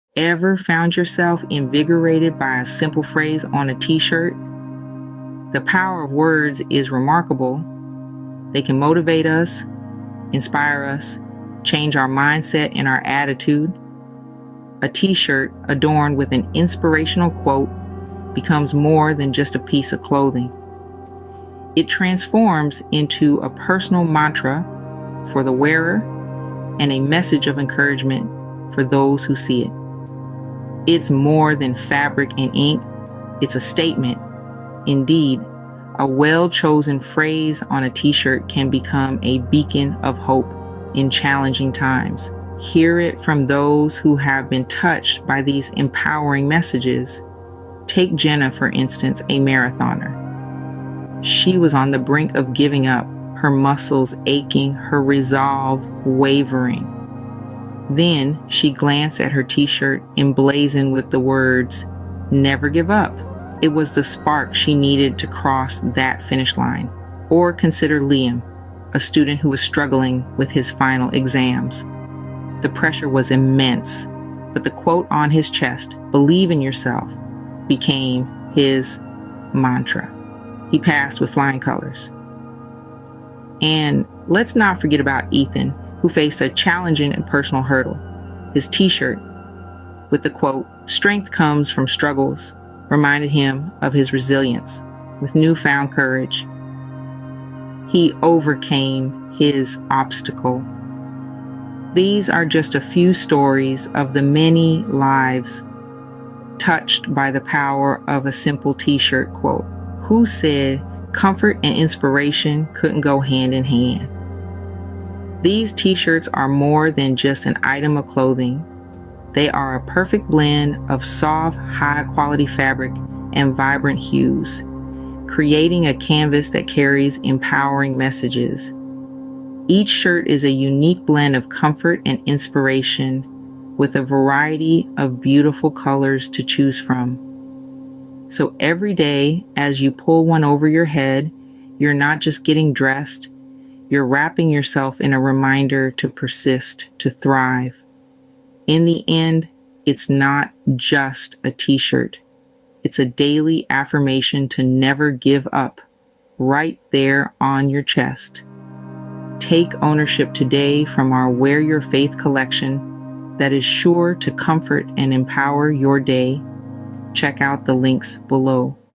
Power of Inspirational T-Shirts
Can a T-Shirt Change Your Outlook? Explore the Never Give Up Tee. In this video, we uncover the stories behind these iconic shirts and how they serve as beacons of hope for those facing adversity. From personal journeys of triumph to collective movements for change, our inspiring tees have become more than just clothing—they're symbols of resilience and determination.

renalsjournalsandmore.shop